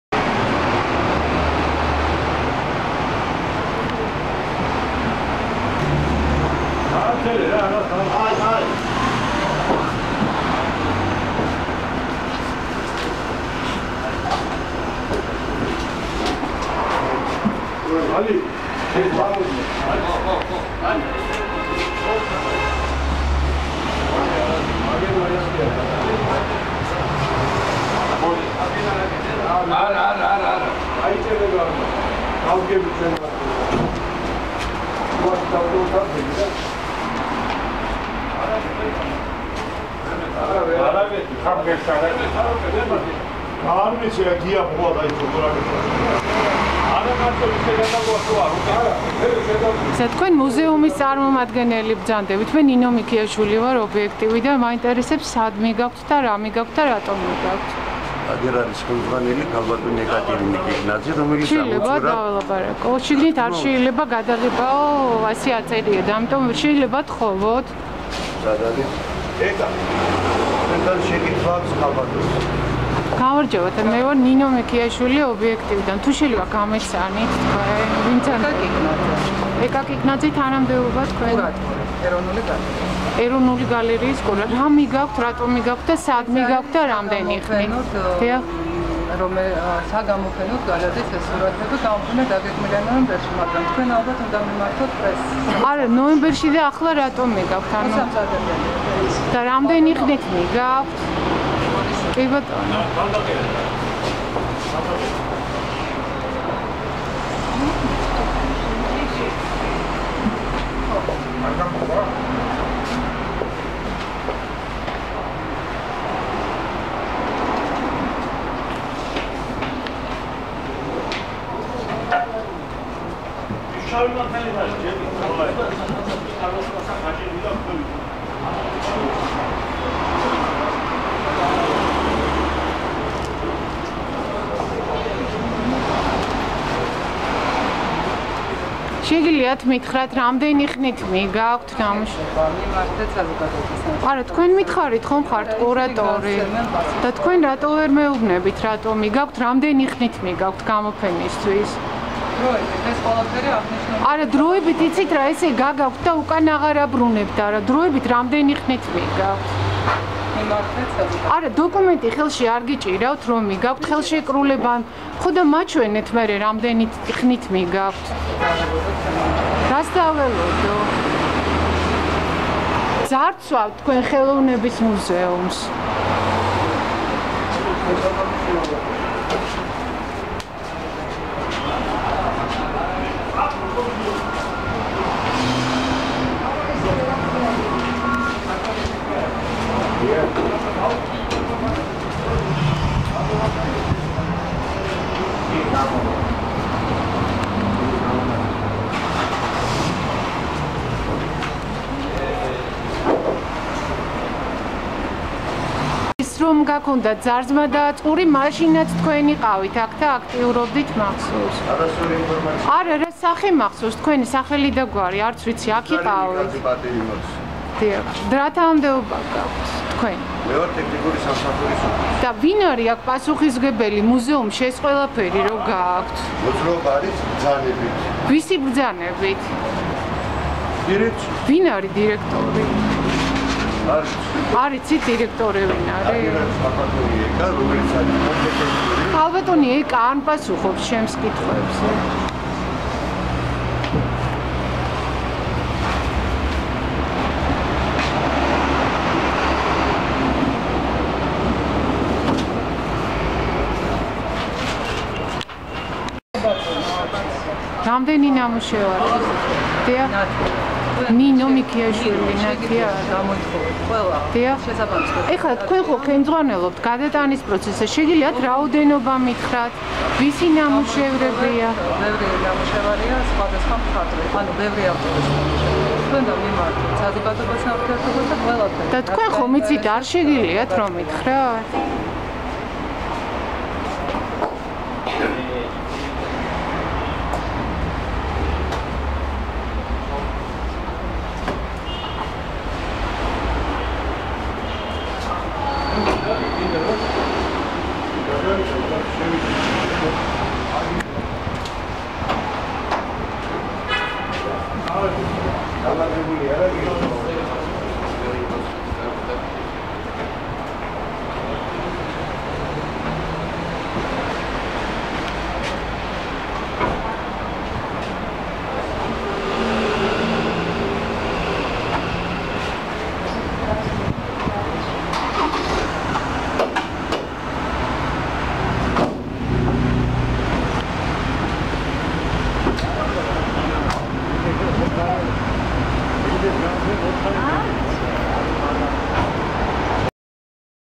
ხელოვნების მუზეუმის ძარცვა 2.10.2014
ნინო მიქიაშვილი: ხელოვნების მუზუემის მორიგი ძარცვის ფაქტი გადავიღე 2014 წლის 2 ოქტომბერს